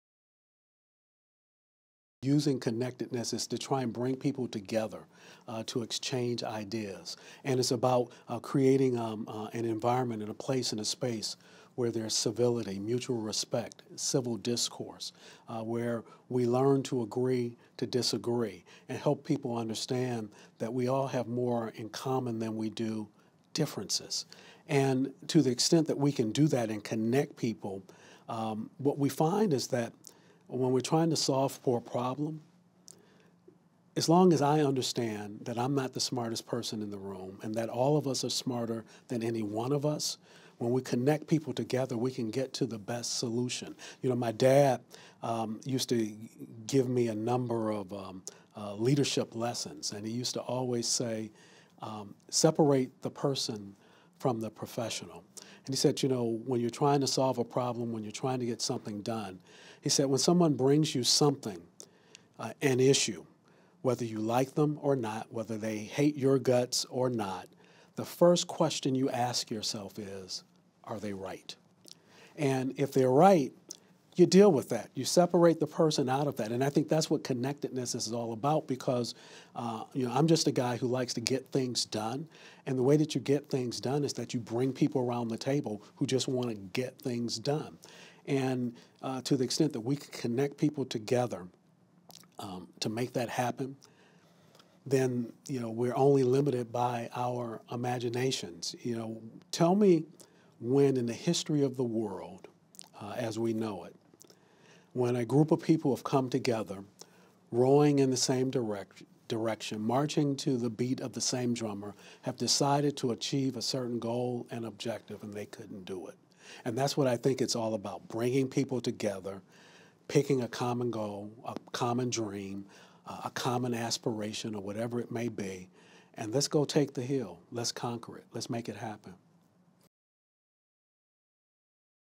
Bringing People Together Through Connectedness | Robert Johnson
Watch Robert Johnson explain the power of Connectedness in creating unity. Discover key strategies for building bridges and fostering a sense of community within your team or organization.